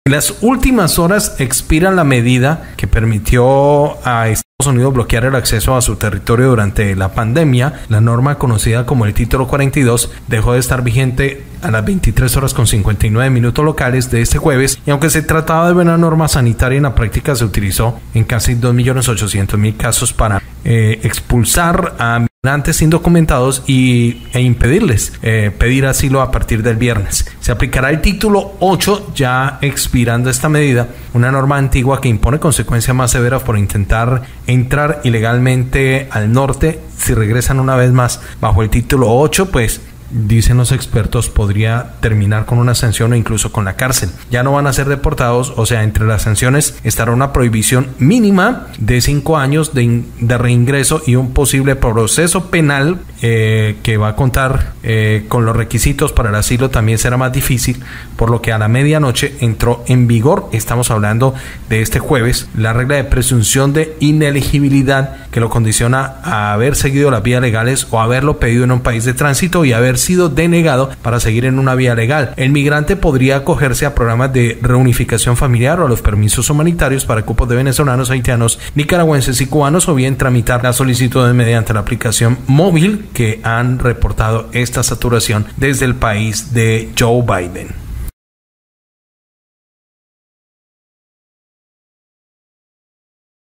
EXPIRÒ EL TITULO 42 EN USA MILES DE PERSONAS ESPERANDO SOLUCIÒN Noticias eeuu 13 mayo 2023.
Lo ùltimo en noticias Internacionales de hoy 13 de mayo de 2023.